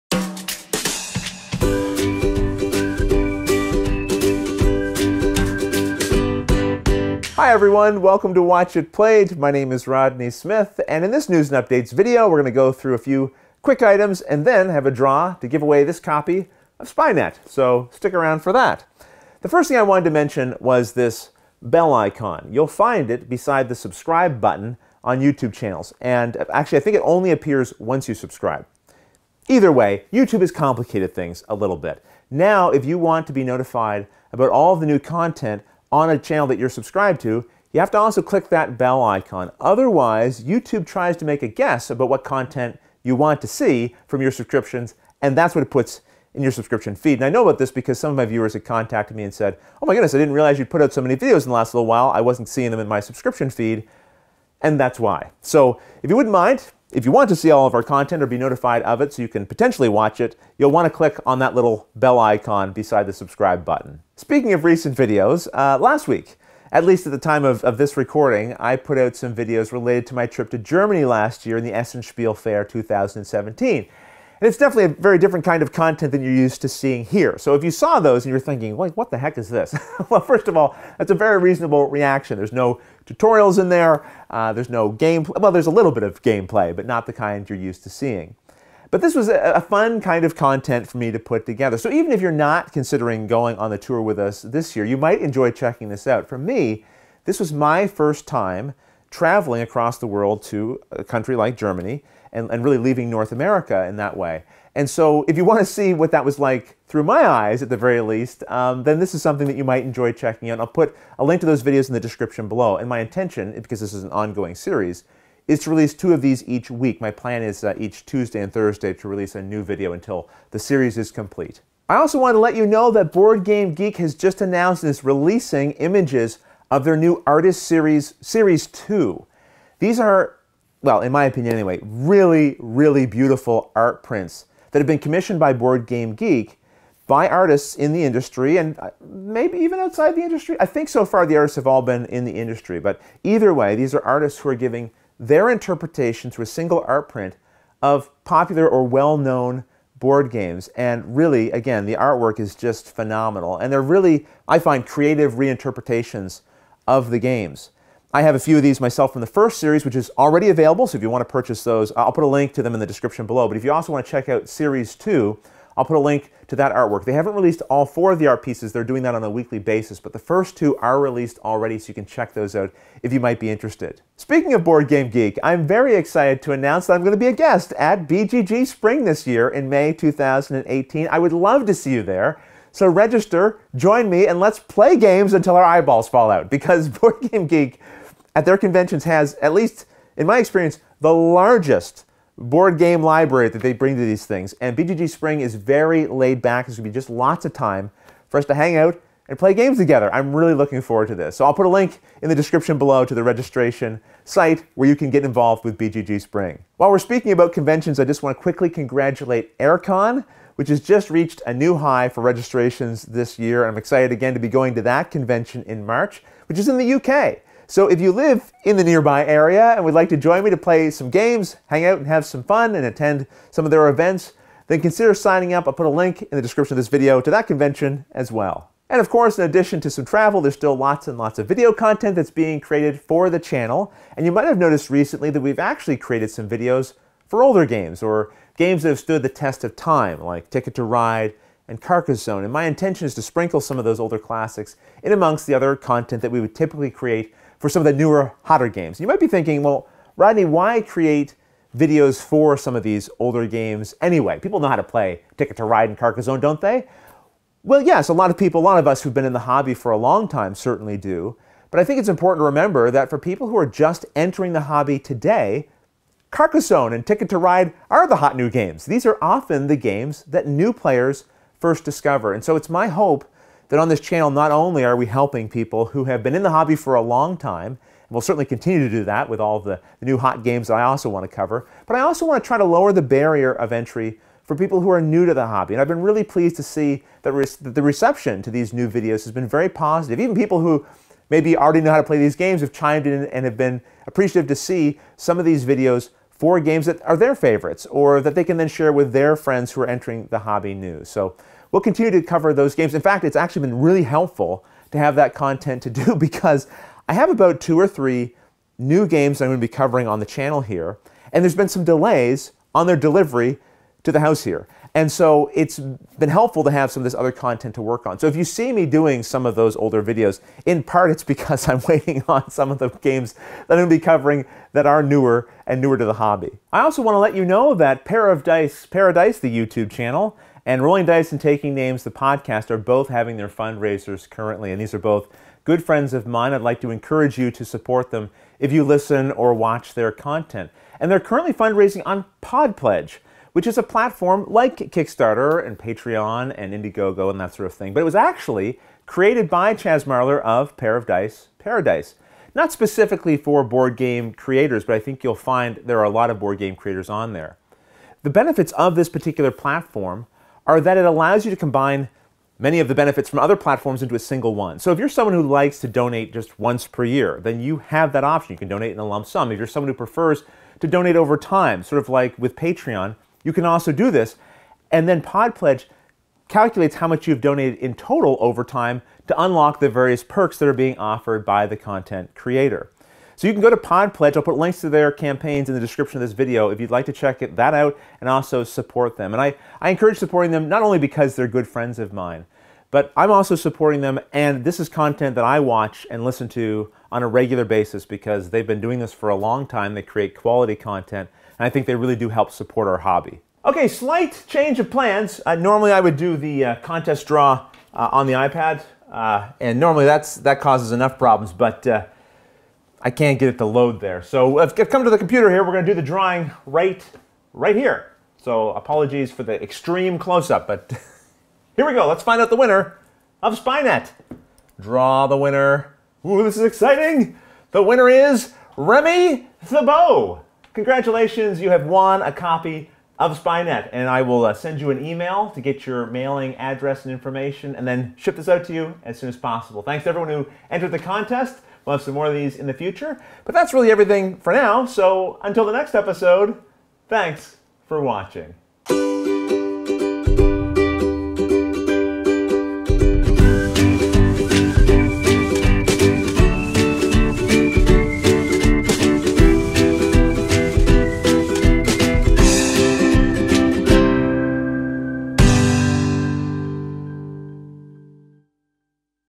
News (2018-02-14): Artists Series / BGG.Spring / Podpledge
Gaming artist prints, BGG.Spring, And Podpledges!

If you have any questions, please do not hesitate to ask a question in the Youtube comments below.